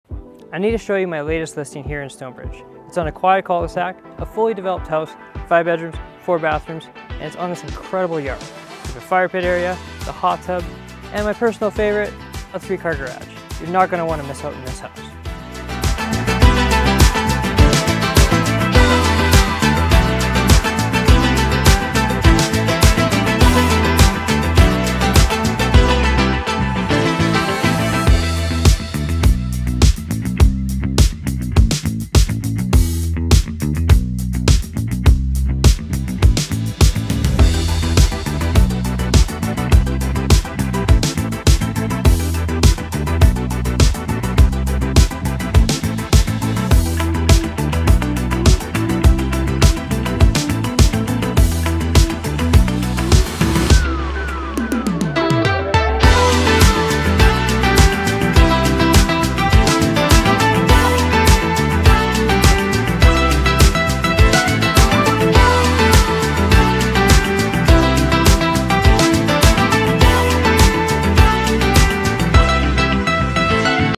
Stonebridge Classic for Sale in Saskatoon
Nestled on a peaceful cul-de-sac, this exquisite 2-story residence sits proudly on an expansive pie-shaped lot, offering both elegance and space. Boasting 5 bedrooms, 3 bathrooms, and a 3-car garage, this home offers you a rare opportunity!
Upon entering, the foyer sets the stage for what's to come. The open-concept main floor welcomes you with an airy layout that seamlessly blends the generously sized kitchen, dining area, and radiant living room. Natural light floods in through large windows, illuminating every corner of this inviting space.
As you ascend the staircase to the upper level, you will enjoy the separation and space. This second floor accommodates 2 spacious bedrooms, a 4 piece bathroom, and the primary bedroom with an 3 piece en-suite bathroom and a walk-in closet. Venturing to the fully developed basement, you'll be met with a world of entertainment possibilities. Two additional bedrooms provide flexibility for guests, a home office, or even a fitness area. The family room, complete with a sleek wet bar, is perfect for hosting gatherings or cozy nights in. A contemporary 3-piece bathroom adds convenience and style to this lower level haven.
As you step onto the expansive deck, the immense backyard unfolds before you. A veritable outdoor paradise, the yard offers space for a hot tub, a dedicated firepit area for gathering under the stars, and even charming planter boxes for your gardening aspirations. Abundant storage options cater to your practical needs, ensuring that every inch of this property is optimized for your lifestyle.
Embrace the luxury of modern living in Stonebridge! Don't miss your chance on this stunning property, call today!